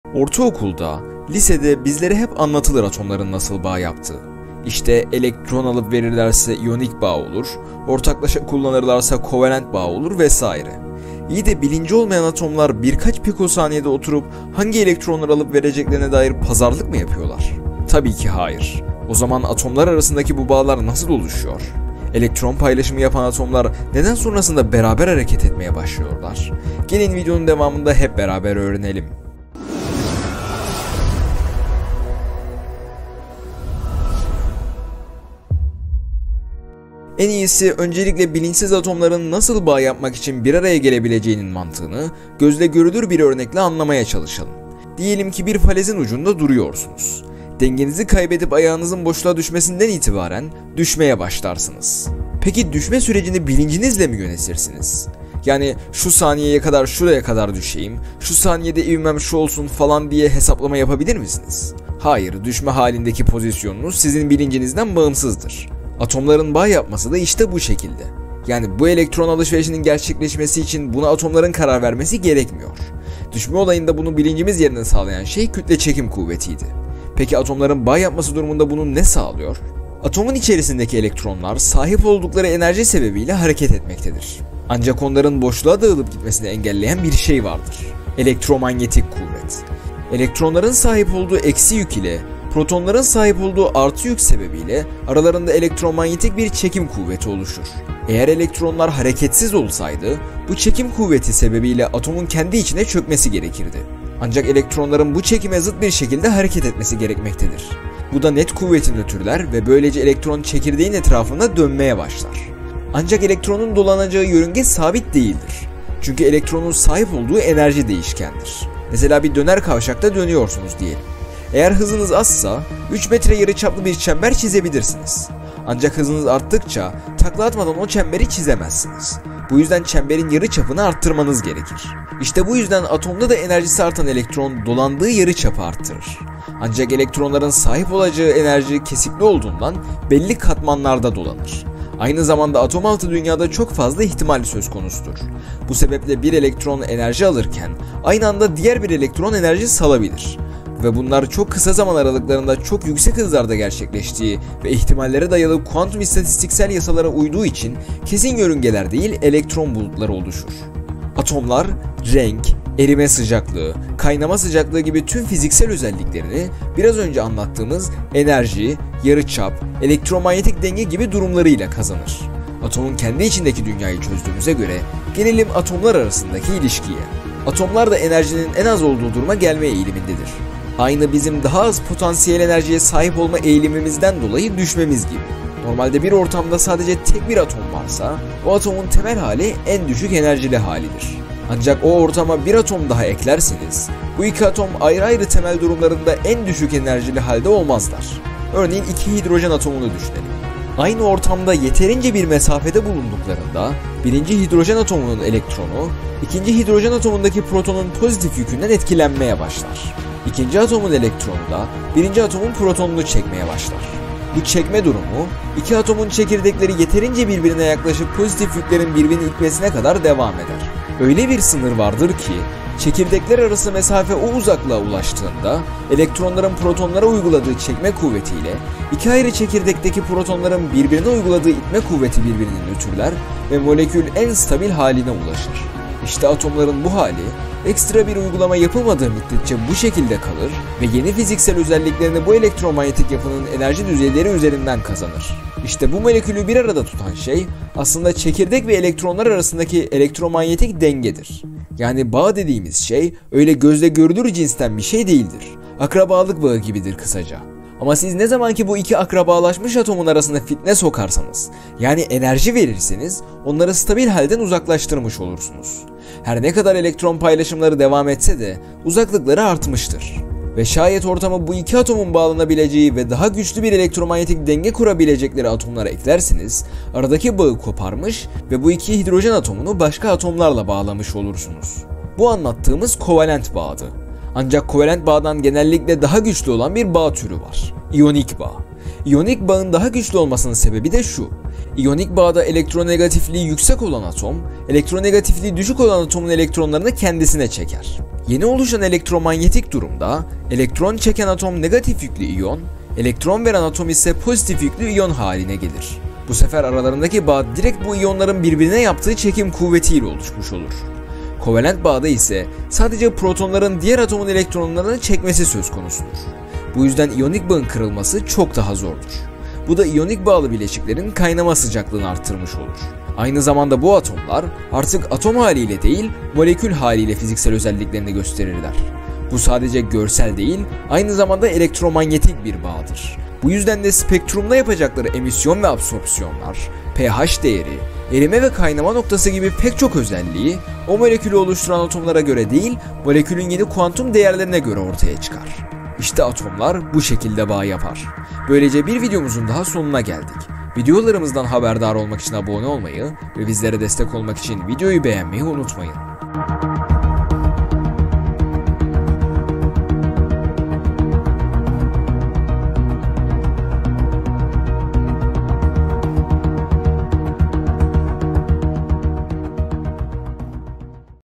ATOMLAR NEDEN BAĞ YAPAR? | ATOMLARIN ELEKTROMANYETİK DAVRANIŞLARI
Ortaokulda, lisede bizlere hep anlatılır atomların nasıl bağ yaptığı. İşte, elektron alıp verirlerse iyonik bağ olur, ortaklaşa kullanırlarsa kovalent bağ olur vesaire. İyi de bilinci olmayan bu atomlar birkaç pikosaniyede oturup hangi elektronları alıp vereceklerine dair pazarlık mı yapıyorlar? Tabii, ki hayır. O zaman atomlar arasındaki bu bağlar nasıl oluşuyor? Elektron paylaşımı yapan atomlar neden sonrasında beraber hareket etmeye başlıyorlar? Kimyasal bağlar oluştuktan sonra oluşan madde neden onu oluşturan atomların özelliklerine benzemiyor? Atomlar tek başlarına durmak yerine neden bağ yapıyorlar? Bu seferki videomuzda atomların elektromanetik davranışlarını inceliyoruz.

Video Planlama ve Metin: Ferkad Kudvetoğlu
Seslendirme: Ferkad Kudvetoğlu
Video Dizaynı ve Montaj: Ferkad Kudvetoğlu
Tasarım: Ahmet Yasin Küçük
Altyazı: Ferkad Kudvetoğlu